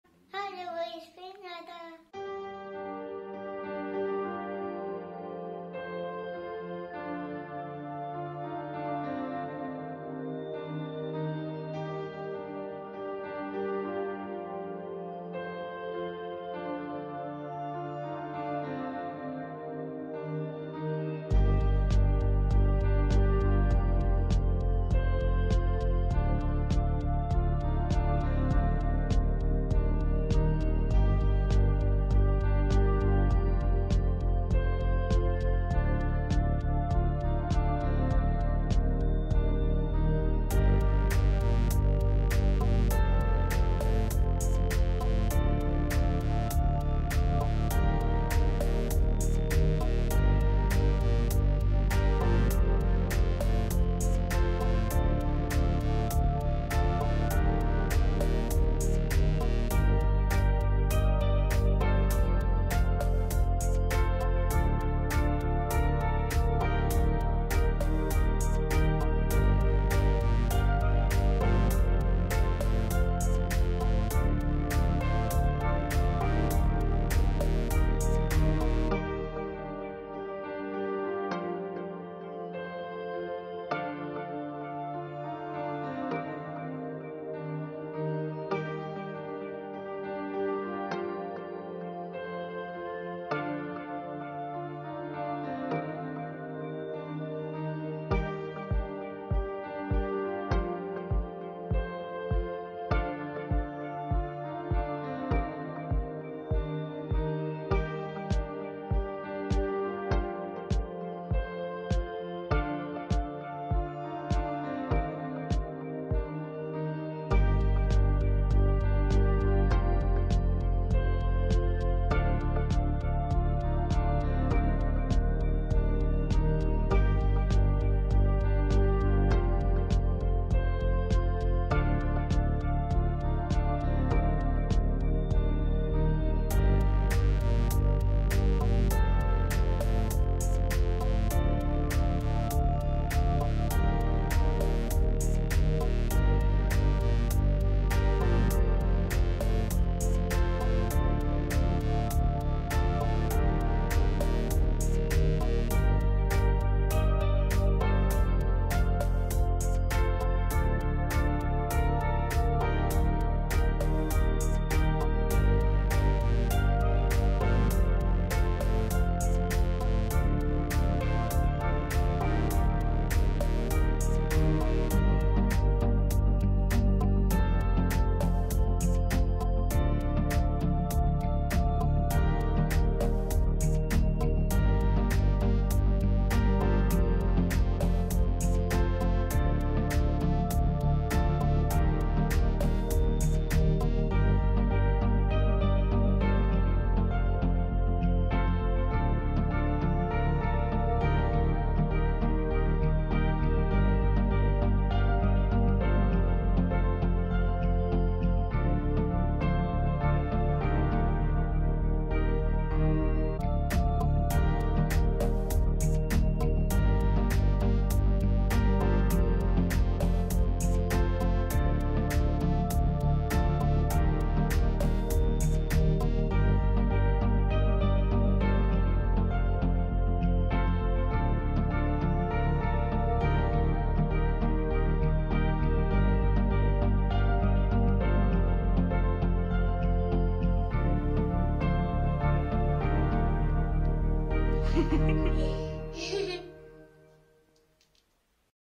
Baby Ada, stretching and flexibility evolution, 3 - 40 months
Baby Ada, flexibility evolution. Skill Compilations 3 - 40 months